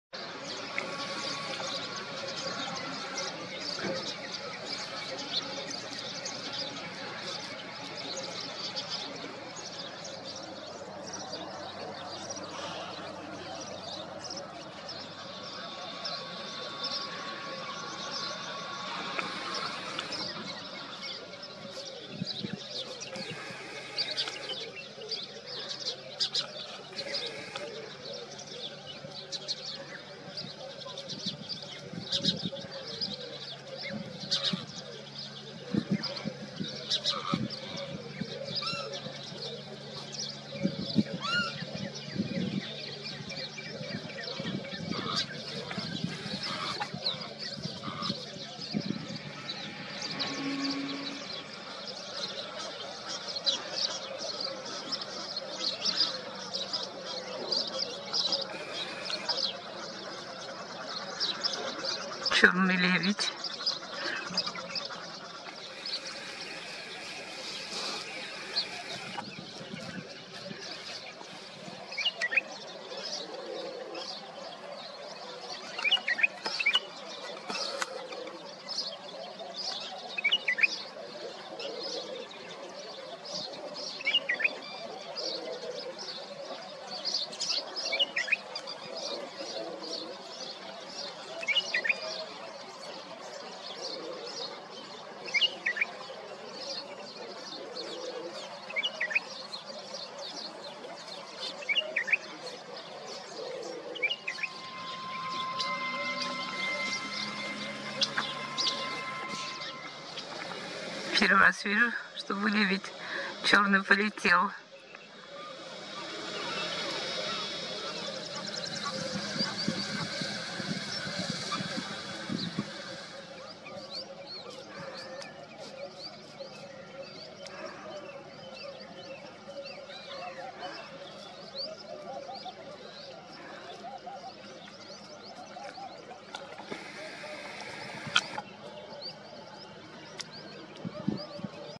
№4674 13 02 2024 Черный лебедь
Черные лебеди в феврале. Подрастают маленькие лебедята. Голос черного лебедя. Полет черного лебедя. Красота и грация птиц.